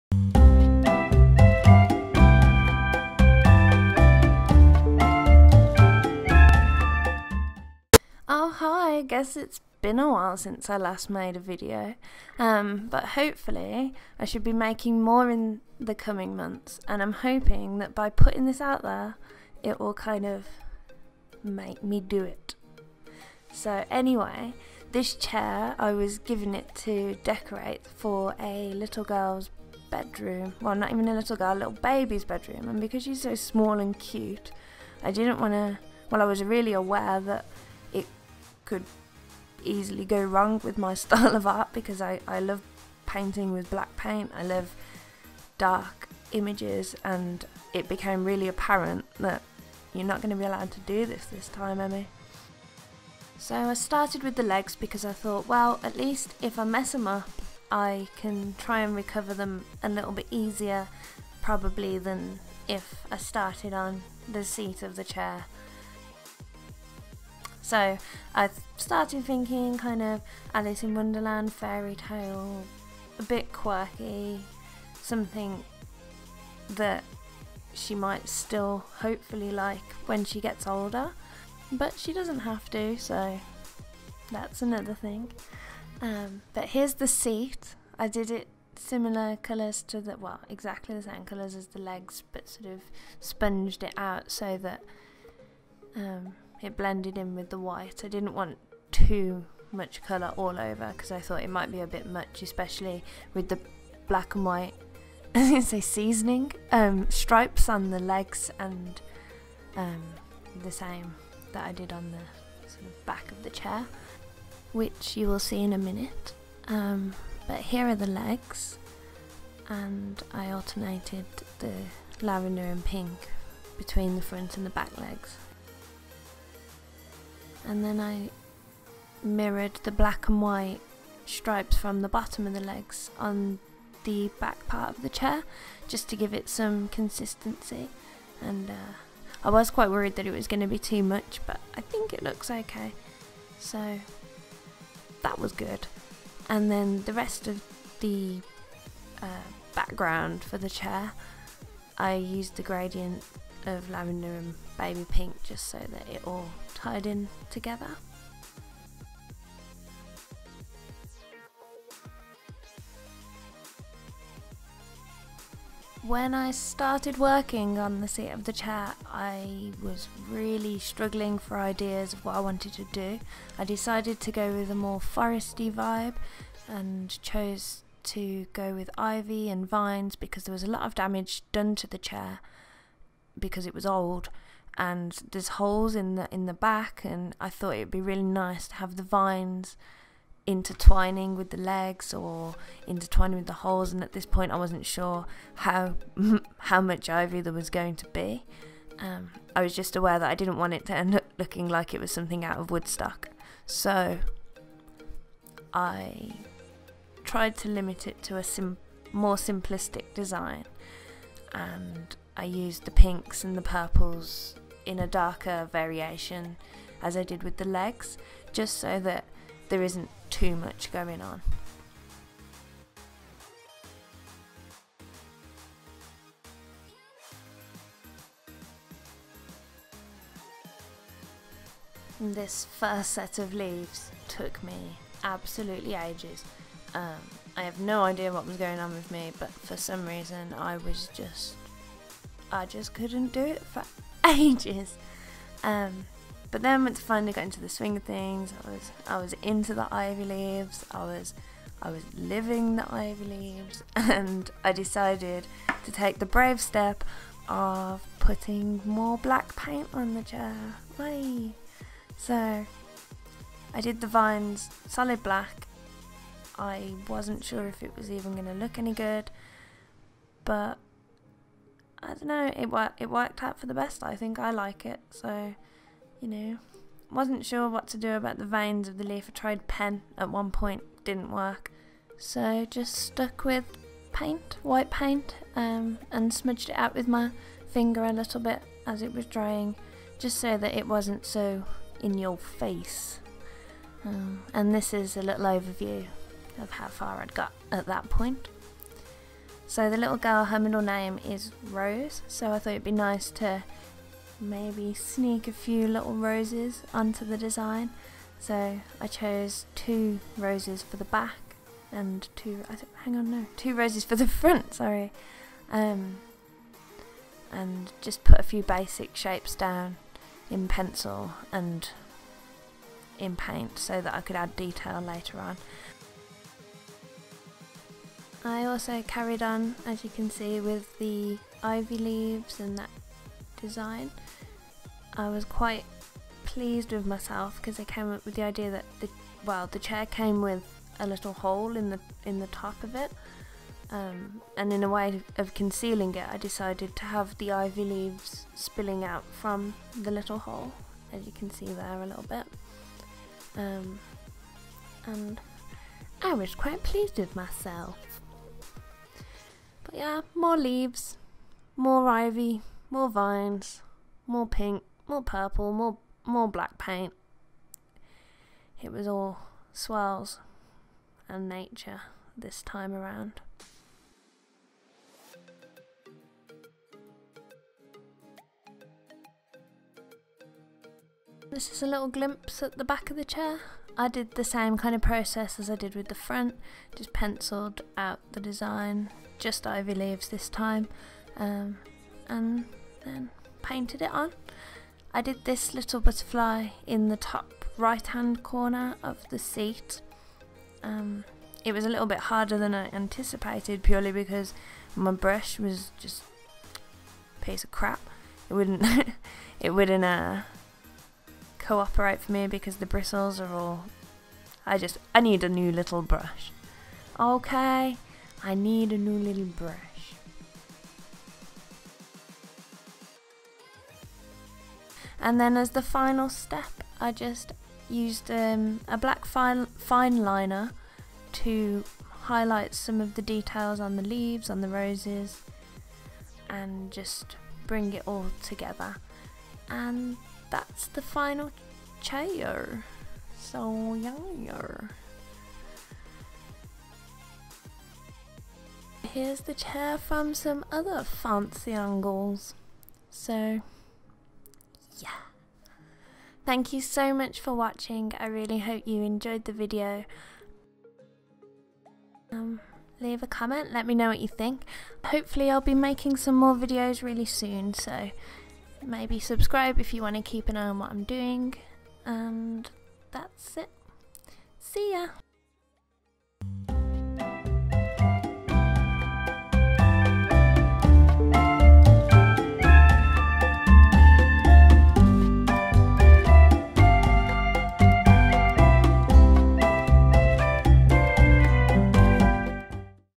HAND-PAINTED CHAIR MAKEOVER | Emy Meowski
Painting a chair for a little girls room! =^-^=